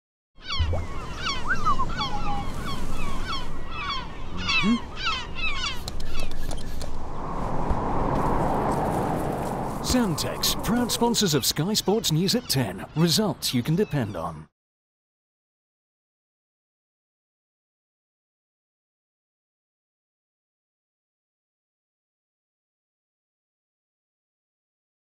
Sandtex 2015 Sky Sports Advert - Football
Sandtex is the UK's number 1 masonry paint brand, part of the Crown Paint family. For a chance to win one of three great sporting prizes,